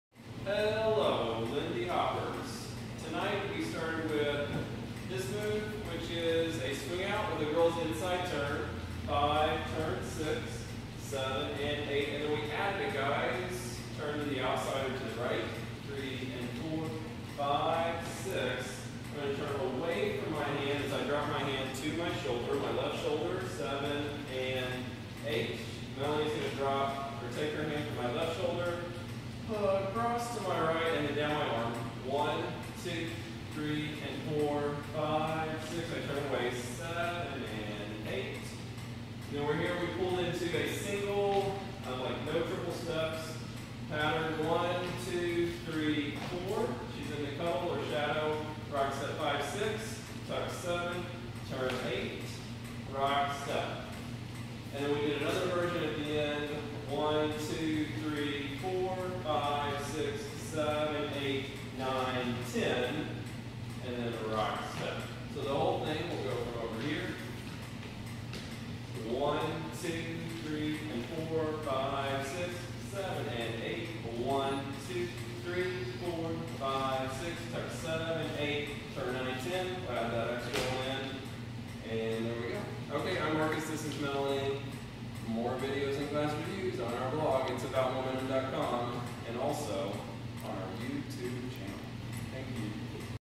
Lindy hop. 6.15.2015. guy's turn from swing out, single step cuddle with tuck turn
lindy hop
marcus coker and melanie medlock

MOVES
swing out with inside turn and guy's turn and hand change
single step cuddle with tuck turn (and extra spin option)